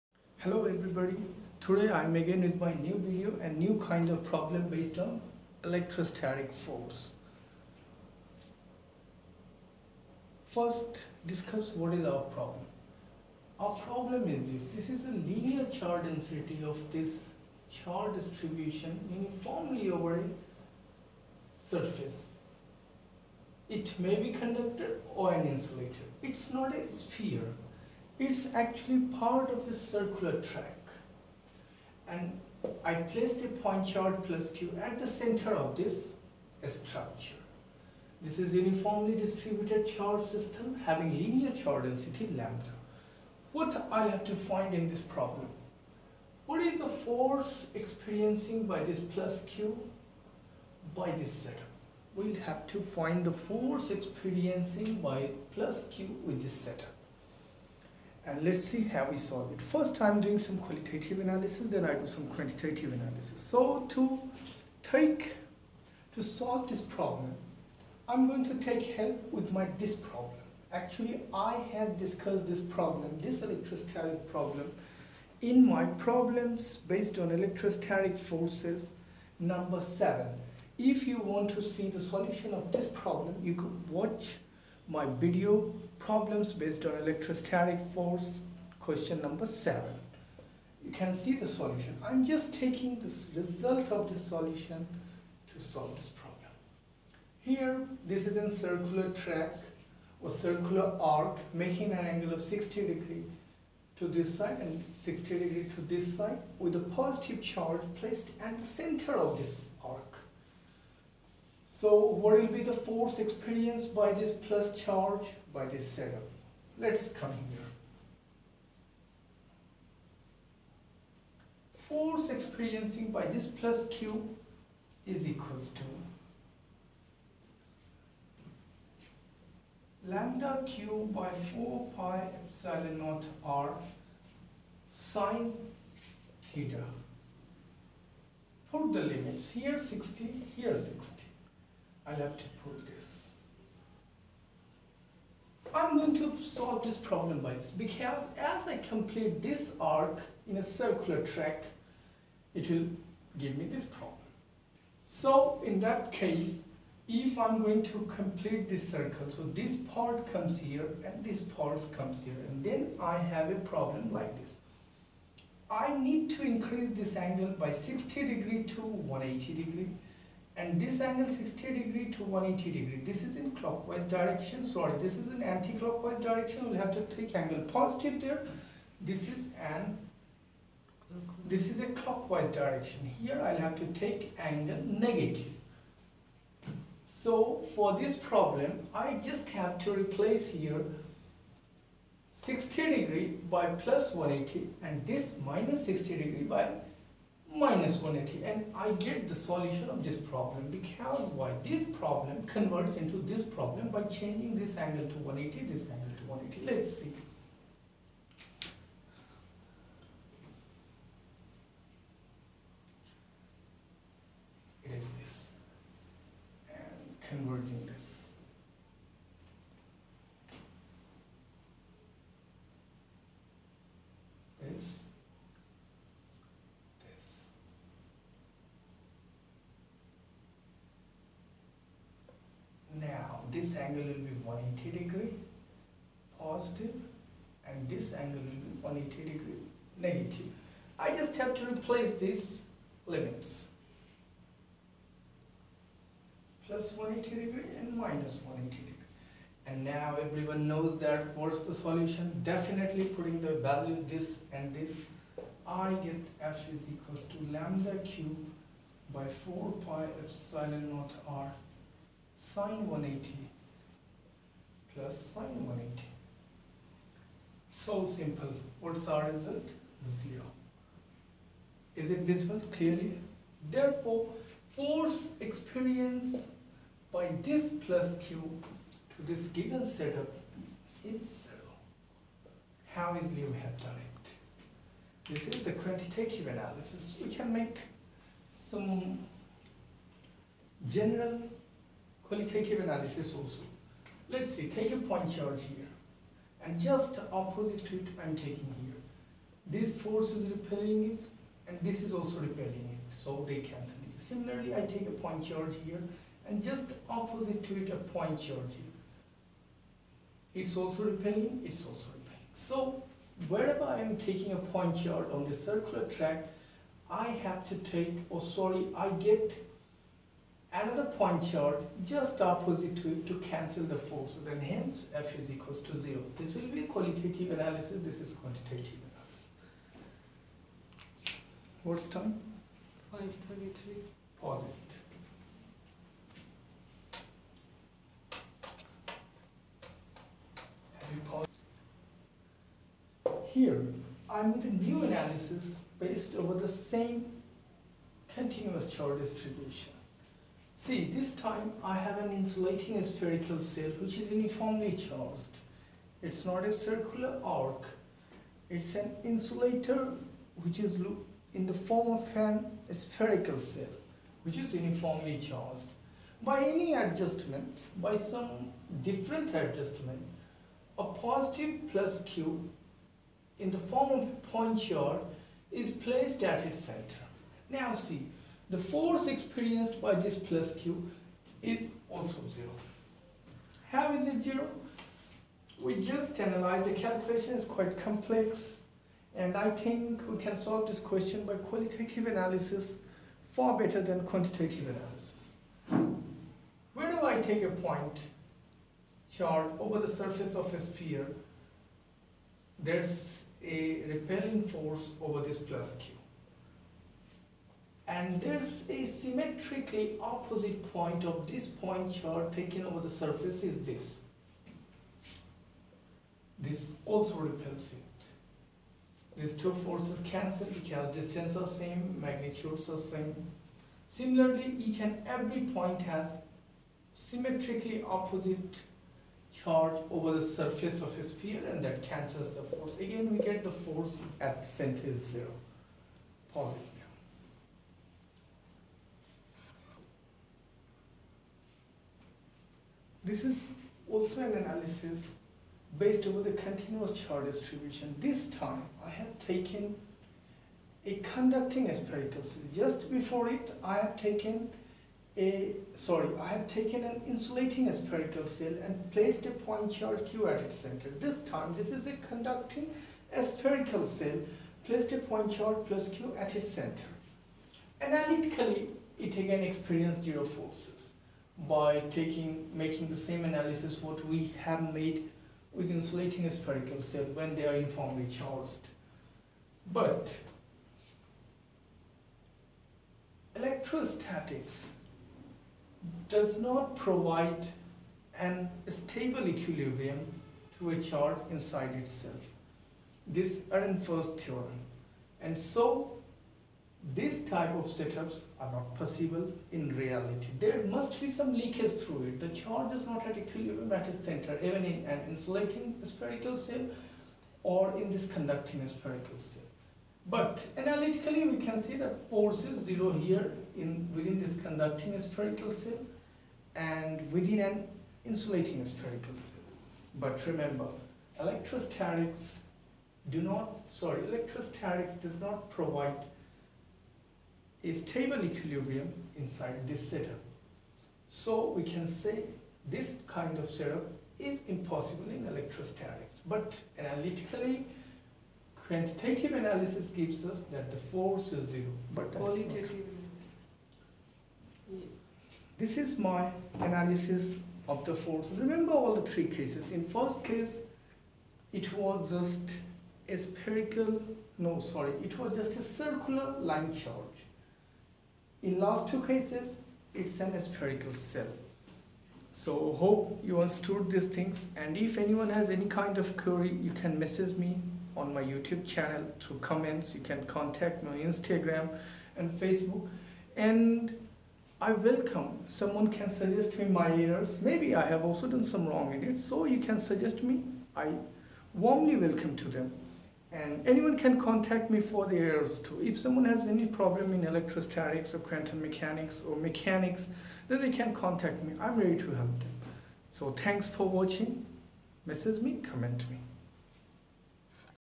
Electrostatic force continuous charge distribution problem-9
The video contains analysis of electrostatic force among a point charge and a continuous charge distribution .I share my view that how to calculate the electrostatic force between a point charge and an arc of continuous charge distribution. I am discussing that how we replace a continuous charge distribution by an equivalent point charge.
We discuss the step by step process to solve this problem.
PHYSICS DEFINES THE WORLD is committed to content.